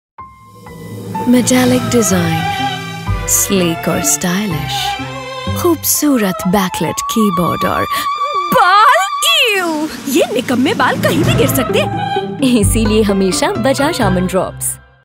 How to Control Hair Fall, Best Oil to Control Hair Fall Daily – Bajaj Almond Drops Hair Oil
Sleek shiny laptops are a dream to work with, only till they are dirty with hair strands! Bajaj Almond Drops Hair Oil is the best option for hair fall reduction! It also promotes healthy hair growth and is known as one of the best hair oils.